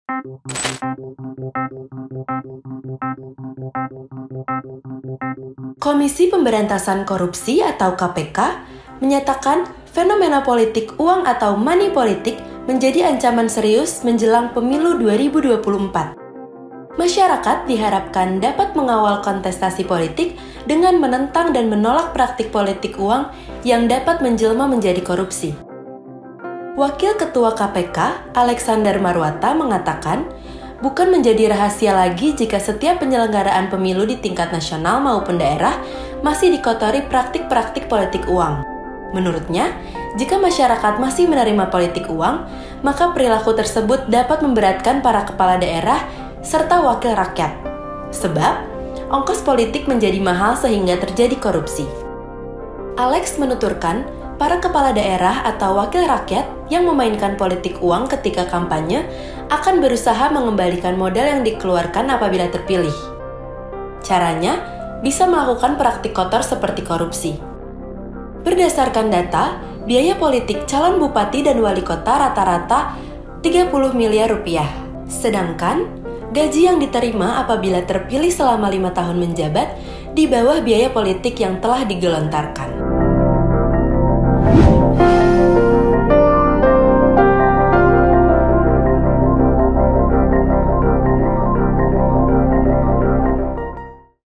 KPK Wanti-wanti Praktik Balik Modal Imbas Ongkos Politik Mahal Calon Bupati-Wali Kota Rp30 Miliar
Komisi Pemberantasan Korupsi (KPK) menyatakan fenomena politik uang atau money politic menjadi ancaman serius menjelang Pemilu 2024. Masyarakat diharapkan dapat mengawal kontestasi politik dengan menentang dan menolak praktik politik uang yang dapat menjelma menjadi korupsi.